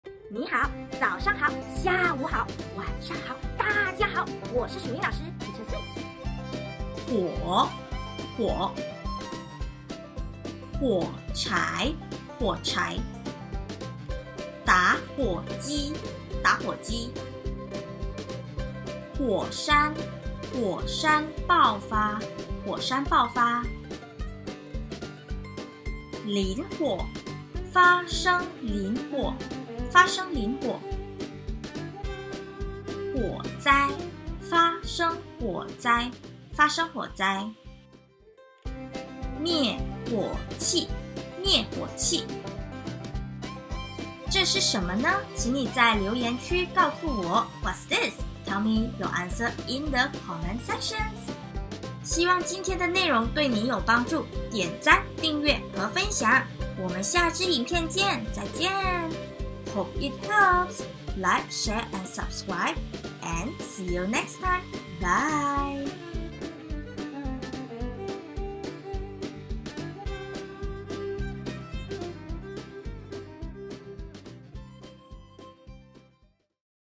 [How to] Fire related in Mandarin “火”相关生词
火 Huǒ Fire
火柴 huǒchái Matches
打火机 dǎhuǒjī  lighter
火山爆发 🌋 huǒshān bàofā  volcanic eruption
发生林火 fāshēng lín huǒ forest fire happens
发生火灾 fāshēng huǒzāi  fire disaster happen
灭火器  mièhuǒqì fire extinguisher

Video/Picture: canva
Editing : Final Cut Pro X